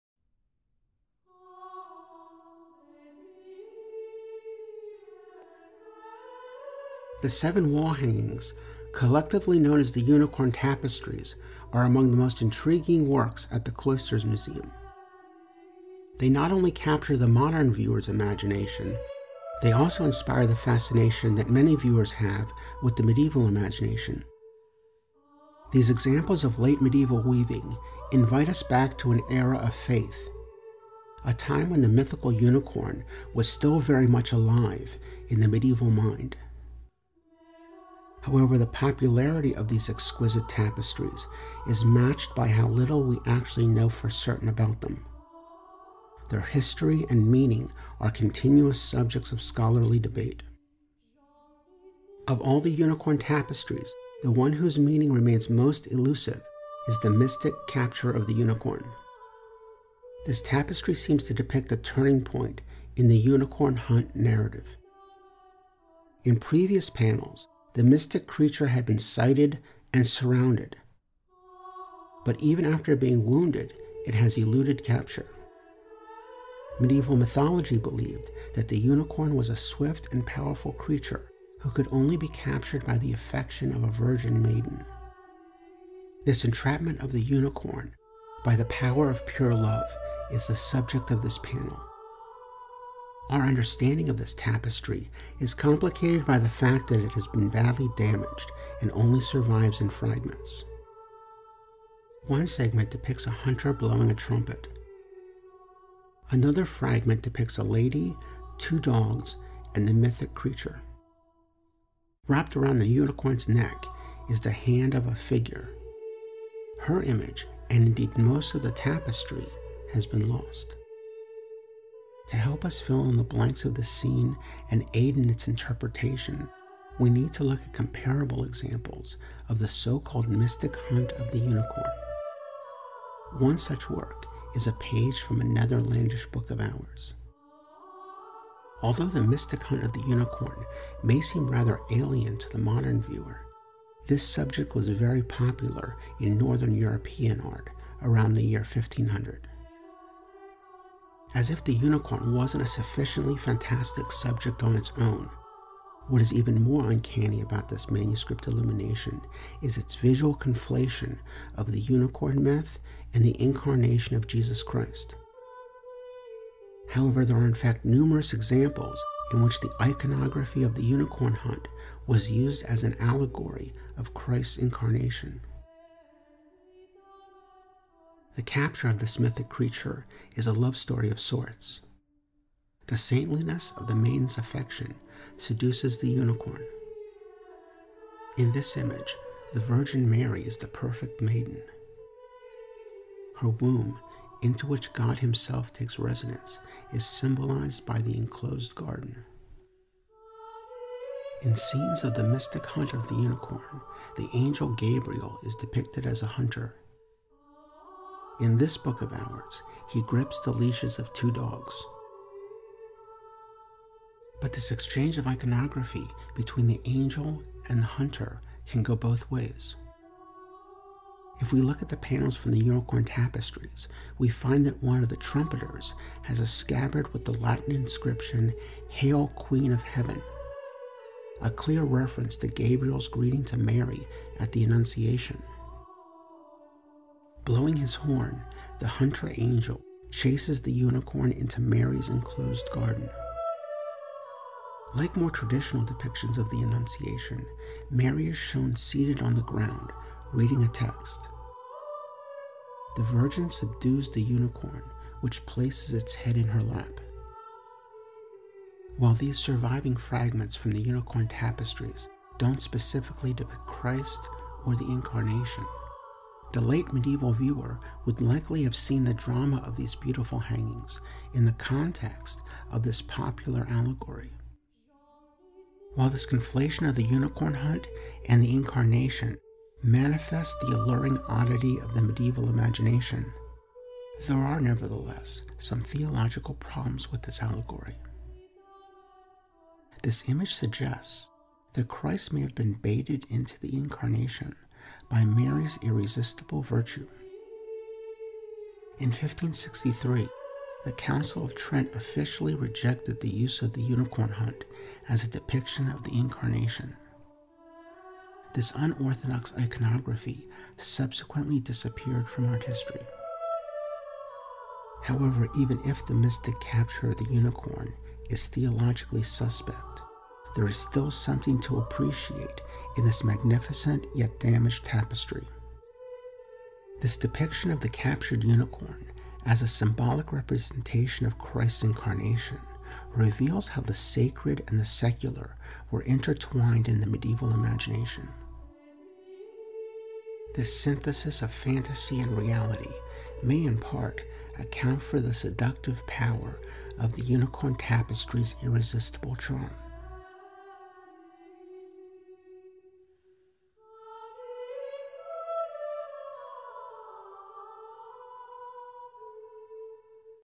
The Unicorn Tapestries: Mystic Capture of the Unicorn
This video this examines the Mystic Capture of the Unicorn. From of the Unicorn Tapestries at the Cloisters, this damaged wall hanging nevertheless highlights the intertwining of sacred and secular themes in the medieval imagination. The iconography of this tapestry is interpreted with the aid of a representation of the annunciation as a unicorn hunt from a Book of Hours. Written and narrated by Dr. James Romaine.